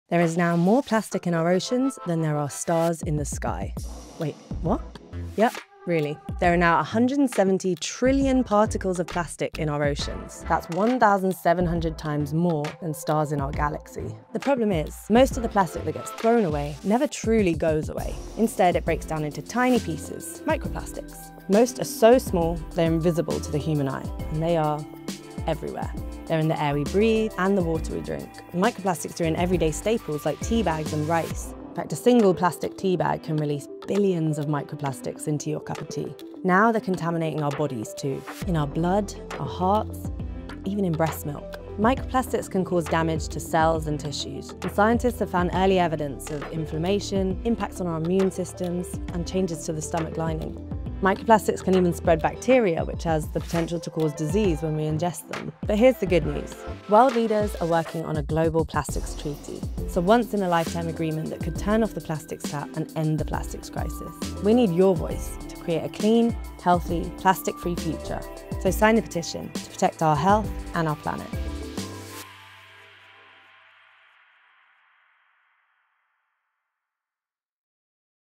The Shocking Truth about Microplastics
🌊👀🥤Did you know there’s more plastic in the ocean than stars in our sky? ….It’s true!

Plastic is everywhere! Microplastics have been found in the 💧water we drink, 🌬️the air we breathe, 🍎the food we eat, and even in 🫀organs and 🩸blood.

Scientists are only beginning to understand the long-term effects of plastics on our health, but what we do know is the only way to reduce microplastics is to cut plastic production via a strong Global PlasticsTreaty.

Watch and share to demand our ministers deliver a strong Plastics Treaty in August!